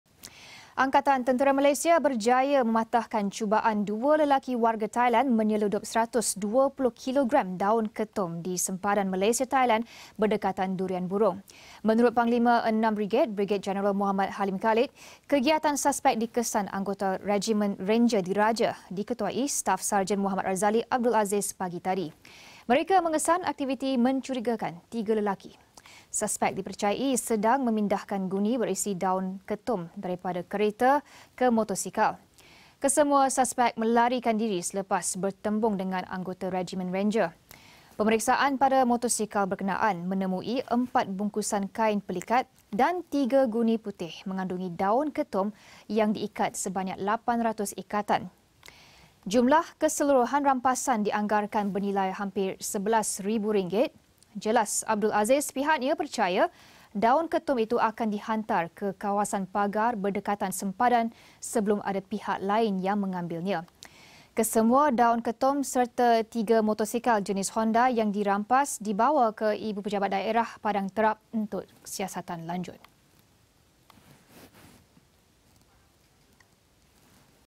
Dua warga Thai ditahan cuba seludup 120 kilogram daun ketum
Angkatan Tentera Malaysia berjaya patahkan cubaan dua lelaki warga Thailand menyeludup seratus 20 kilogram daun ketum di sempadan Malaysia-Thailand berdekatan Durian Burung.